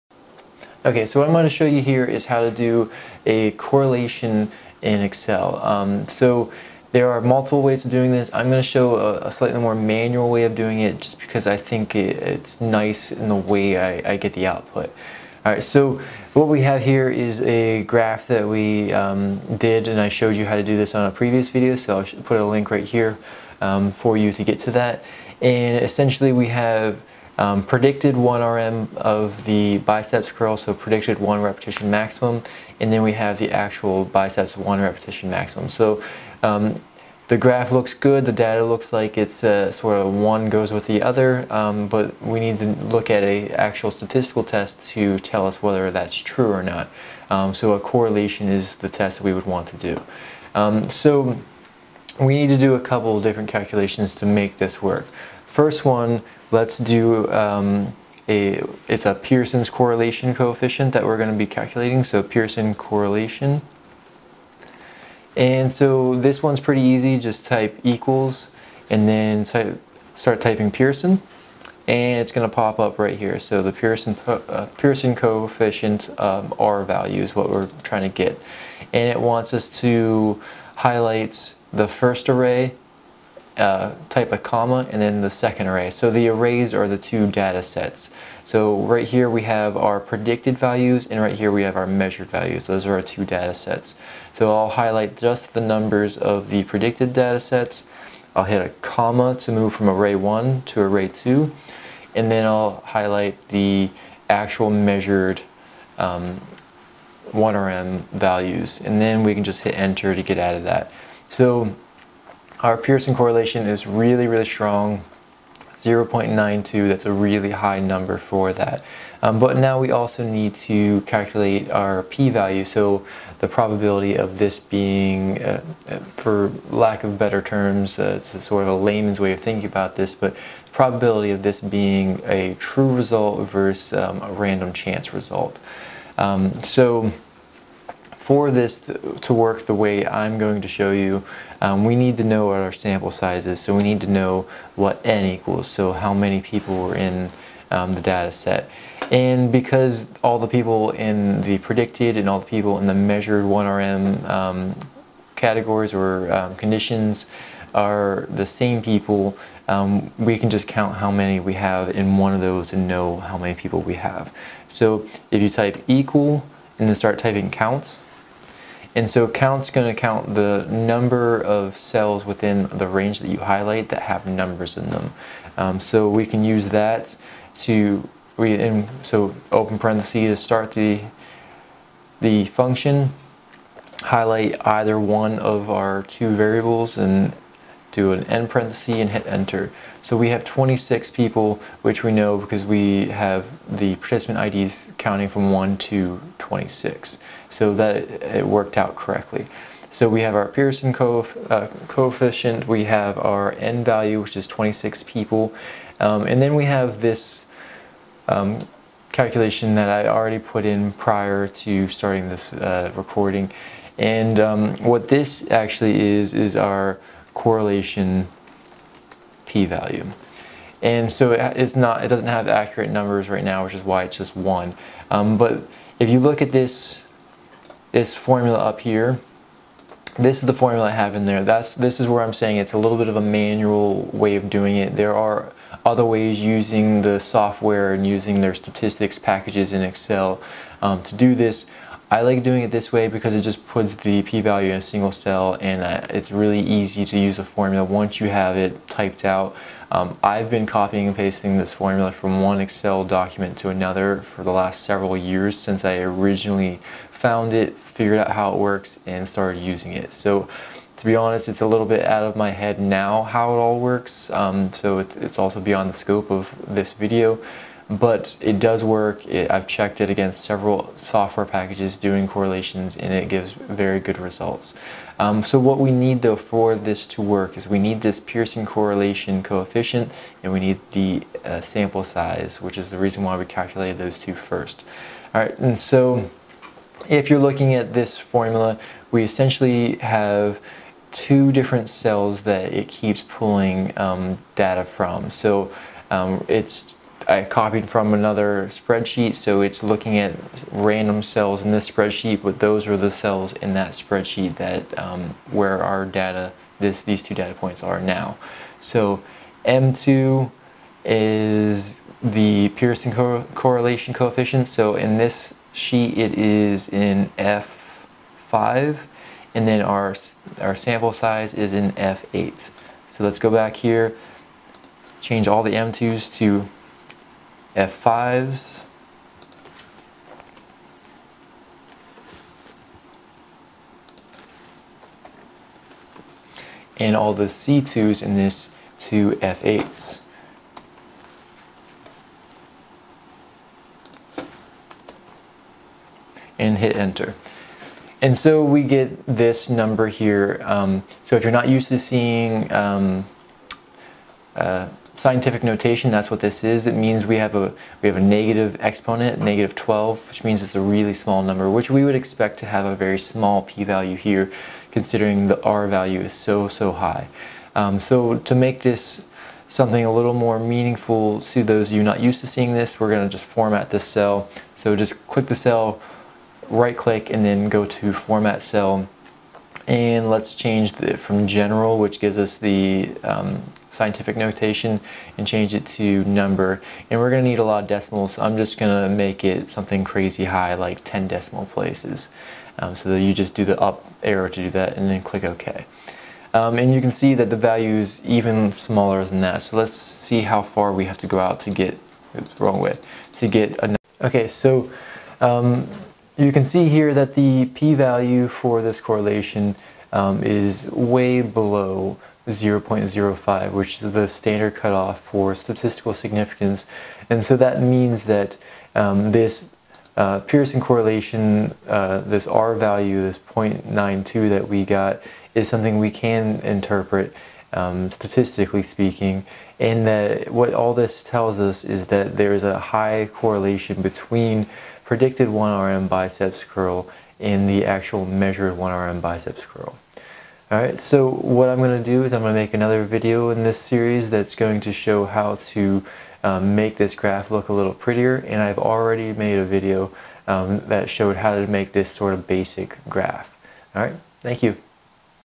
Correlation Statistics in Excel Basic (NEW VERSION and CORRECTION IN DESCRIPTION)
NEW VERSION

CORRECTION:
In the video I used the following formula to calculate p values (=TDIST(ABS(([rValue]*SQRT([nSize]-2)/SQRT(1-([rValue]*[rValue])))), [nSize], 2)). The last [nSize] is the portion of the excel function where the degrees of freedom is entered. This should have been [nSize]-2. I accidentally left out the -2. Fortunately, it makes minimal difference with typical sample sizes. My quick testing suggests that it only matters at the 3rd decimal place with sample sizes of at least 10. Once over 20 participants it is a small enough difference that it would not impact reported data as the difference would be rounded away. Regardless, the formulas given below have been corrected.

This video shows Dr. Evan Matthews explaining how to perform a correlation in Excel, and how to interpret those results. The example data in this video represents the predicted 1 repetition maximum of the biceps curl and measured 1 repetition maximum of the biceps curl.

Example filled in formula for correlation from video:

=TDIST(ABS((F5*SQRT(F8-2)/SQRT(1-(F5*F5)))), F8-2, 2)

Link to video showing how to improve the appearance of this graph to mimic the appearance of graphs typical in physiology research papers.